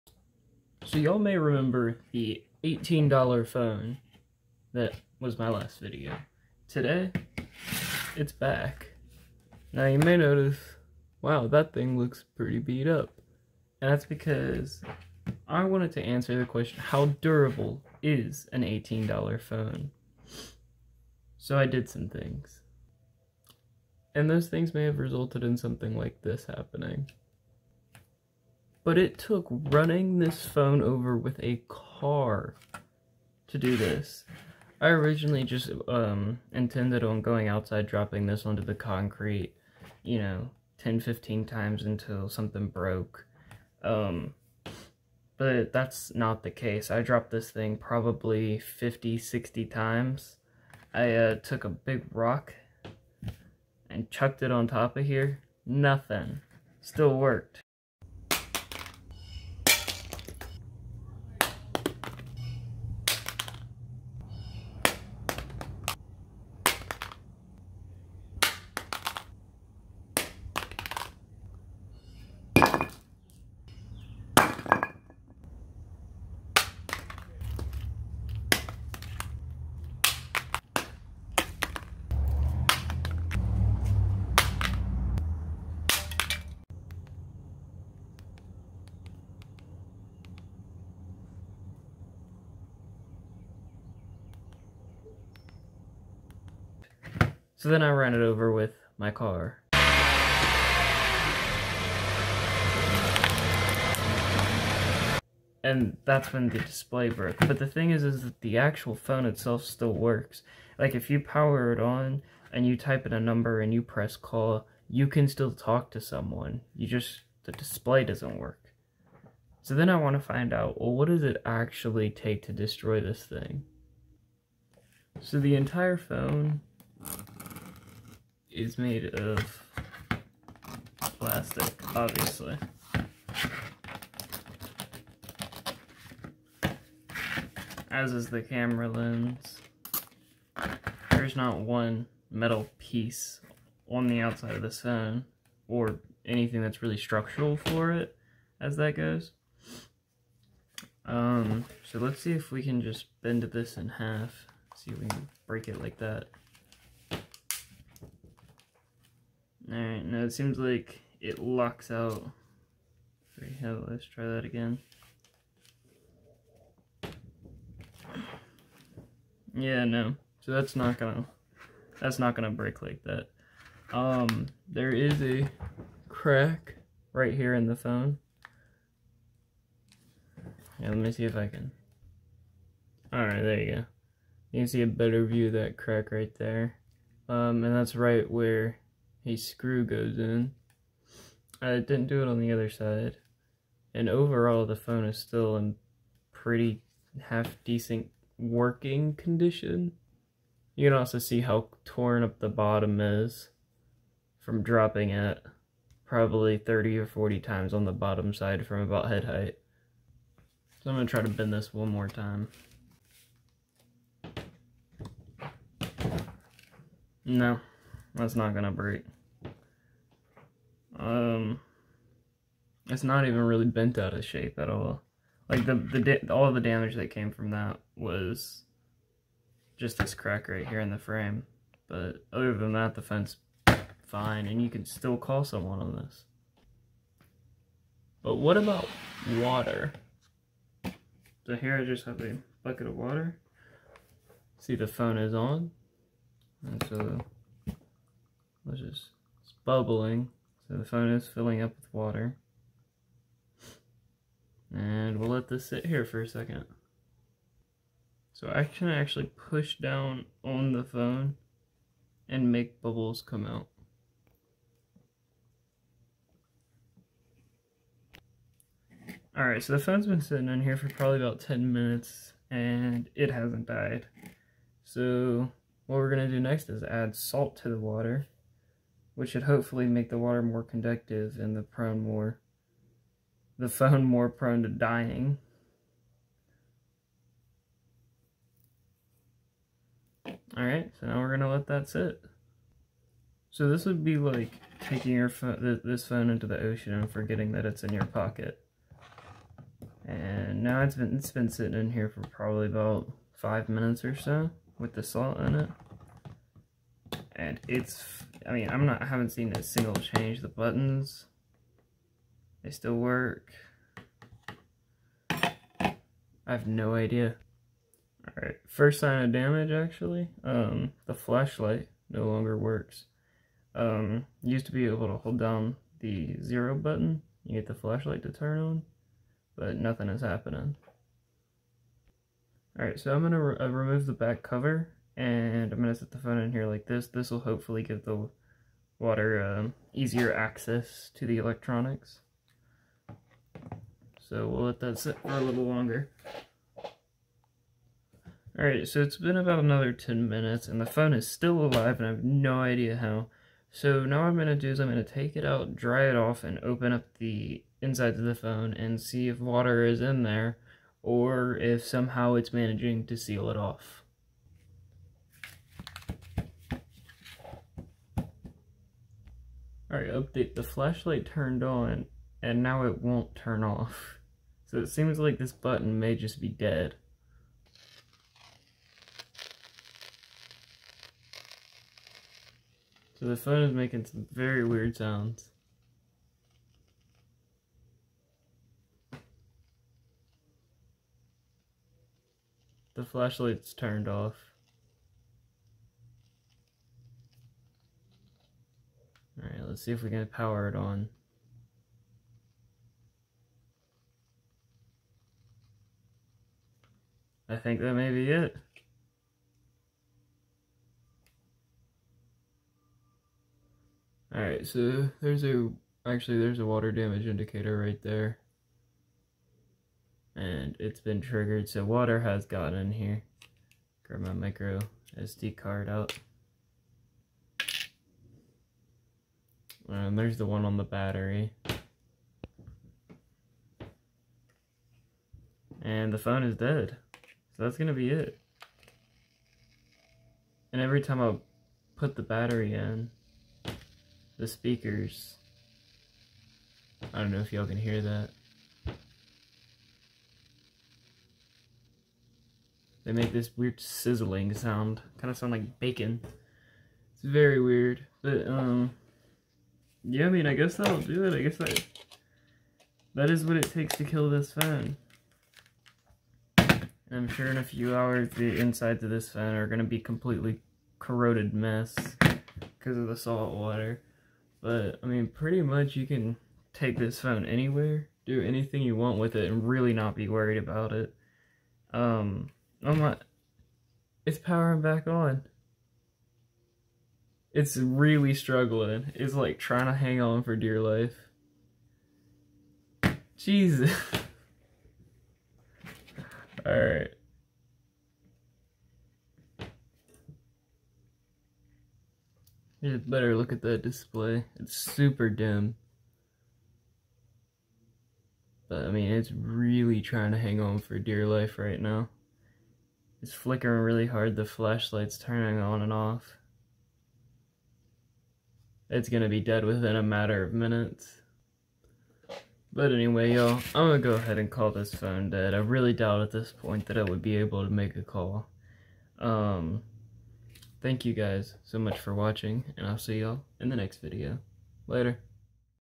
Blu Z5 durability test
Blu Z5 durability test. Watch to find out how durable this little 18 dollar phone is.